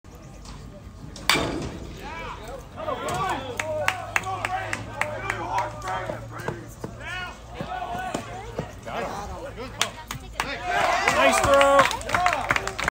William Monroe HS (2025) 1B/OF Waylon Anderegg drills a 2 RBI single up the middle.
William Monroe HS (2025) 1B/OF Waylon Anderegg drills a 2 RBI single up the middle for Valley Elite 2025 McCoy.